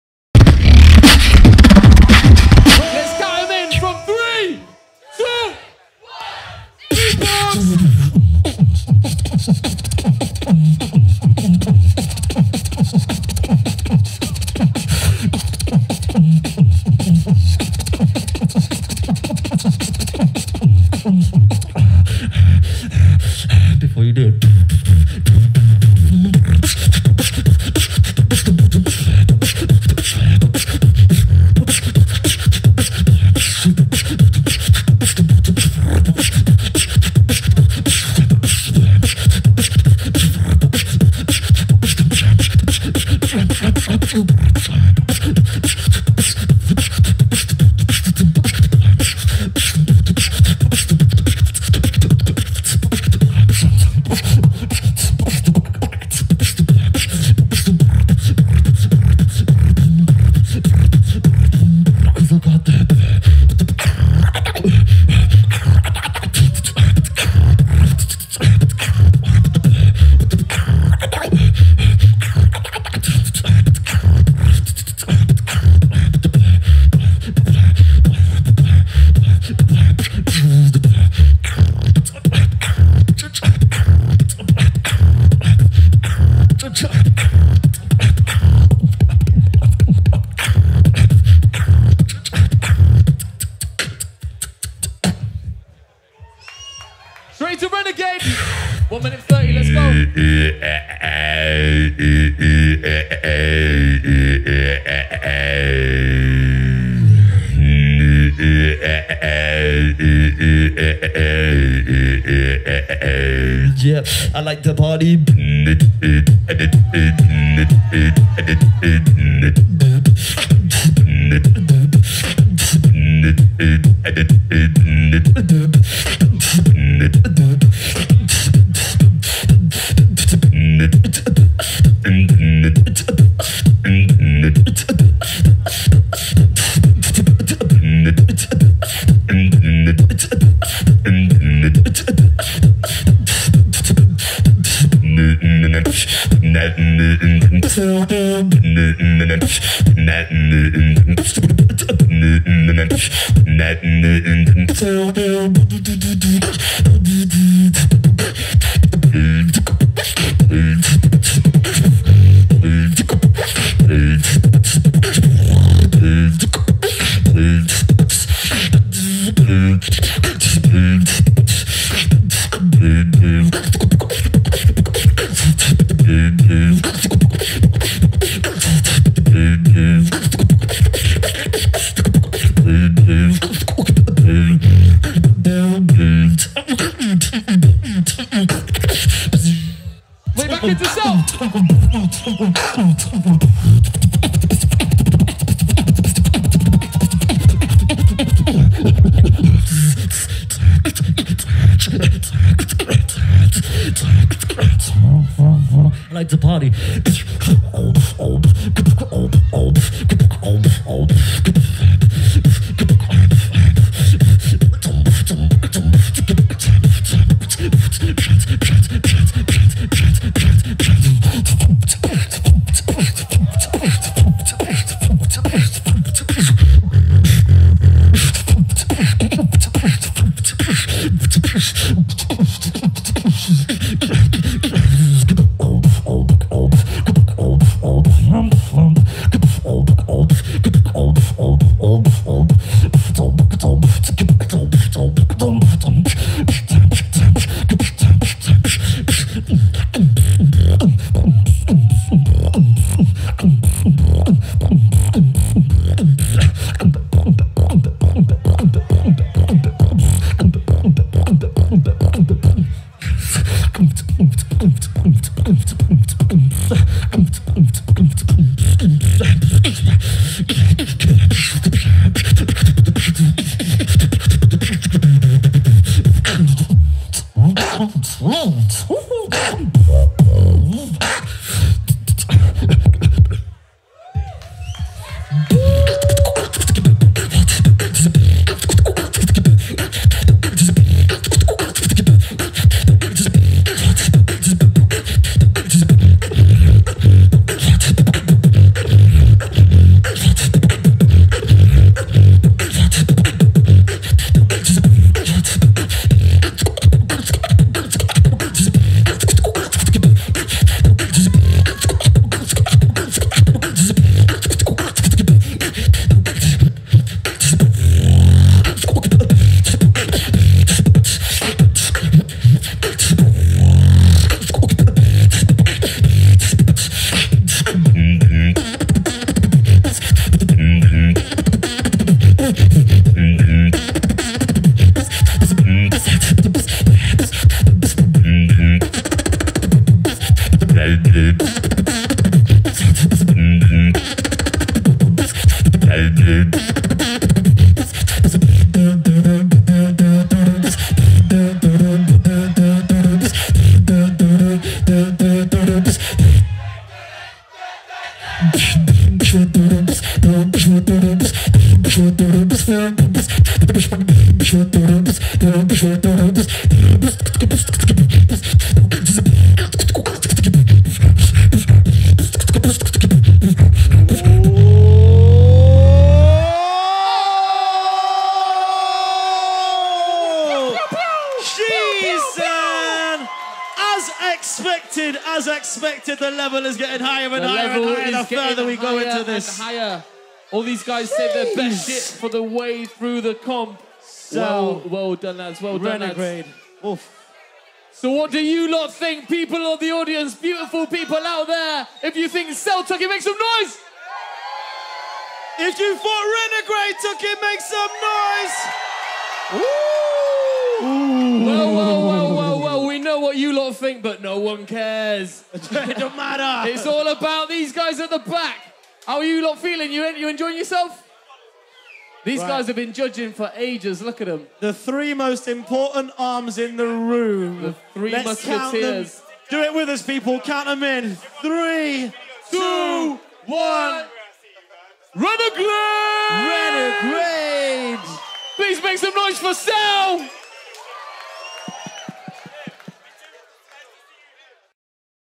Sel vs Renegrade | Solo Quarter Final | 2017 UK Beatbox Championships
A 5th Element, Lyrix Organix production, presented by POPBOX.TV

Video and editing by Broken Toy Films.